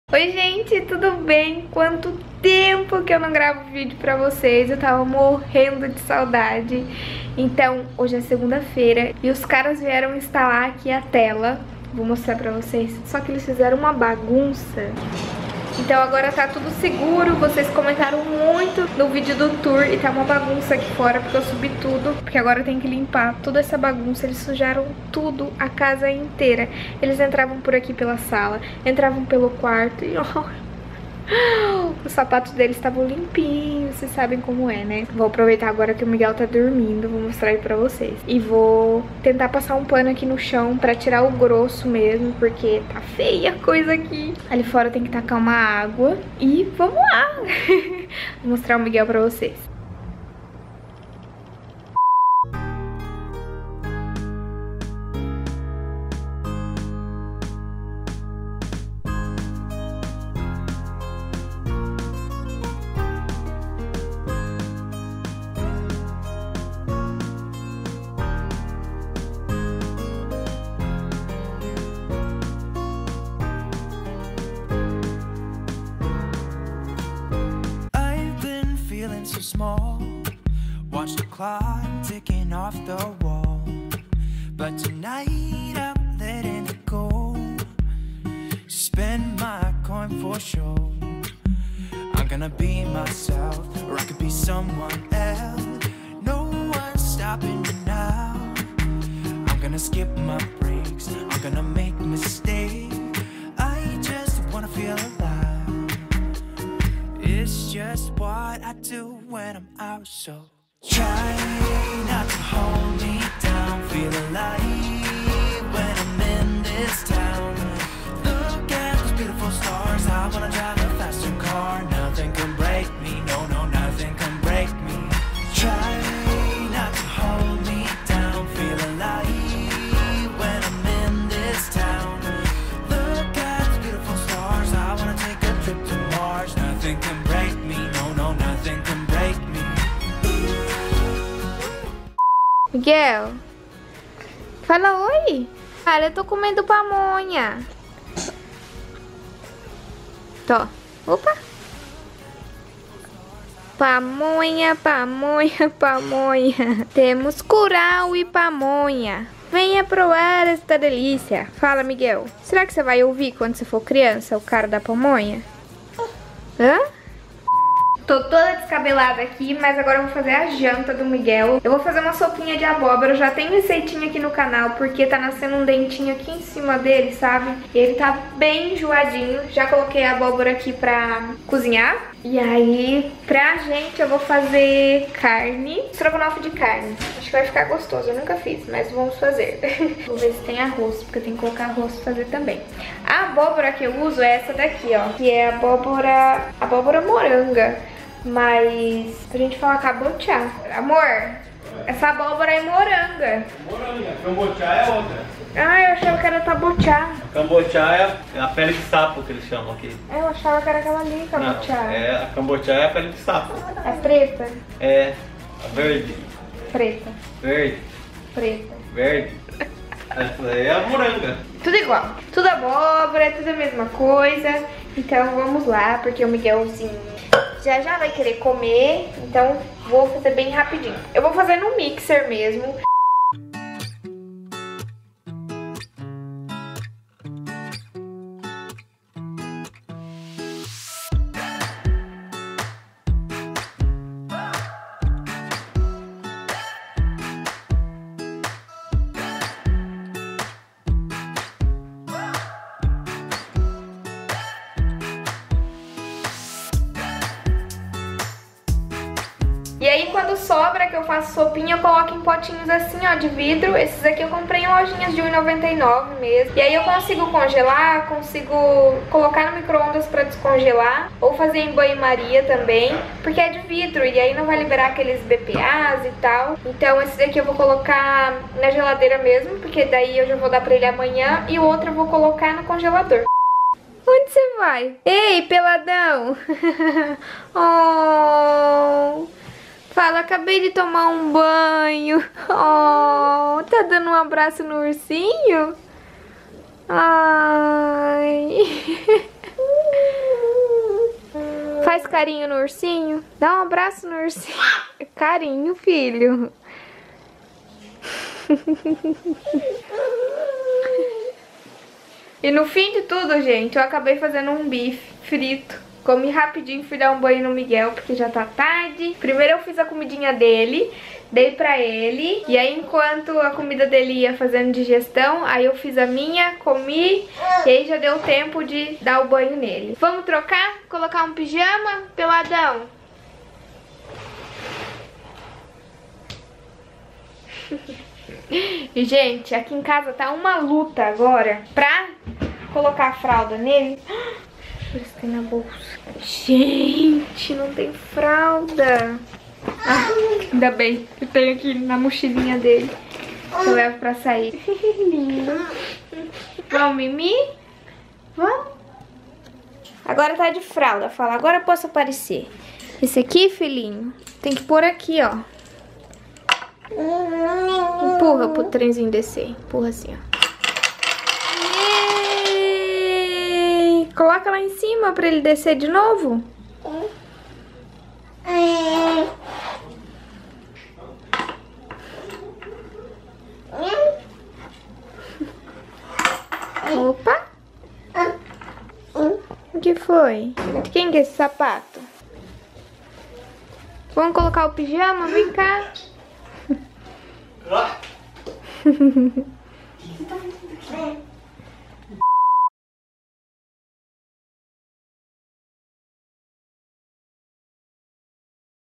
ROTINA DIARIA - VANESSA MORAES
Oi gente, hoje vieram instalar a tela de proteção aqui no apê e fizeram uma bagunça, então vim mostrar um pouquinho de como eu limpo tudo por aqui.
Por fim fiz uma sopinha bem gostosa pro baby Miguel por que agora com os dentinhos crescendo ele quase nao come muito ..
E a sopinha ele adora :)

SNAP -VanessaMOficial